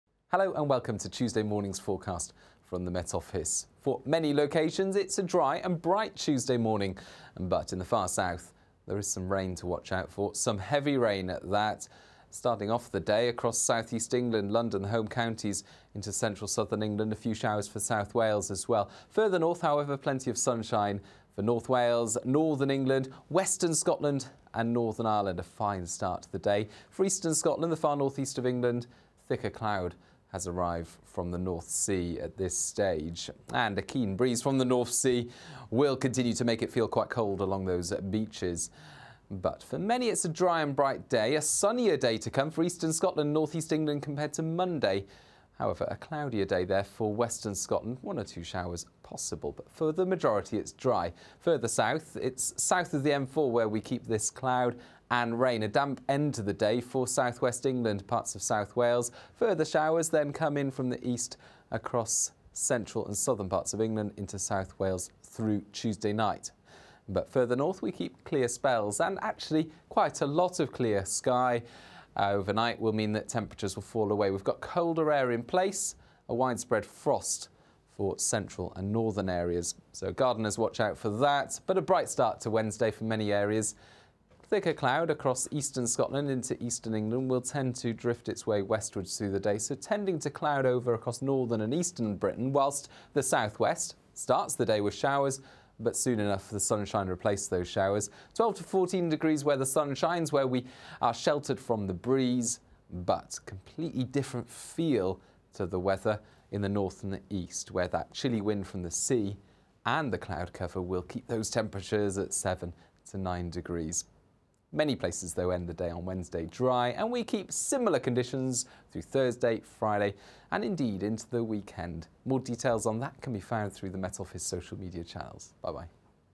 Tuesday morning forecast 09/04/2019
9 April - National weather forecast presented by Aidan McGivern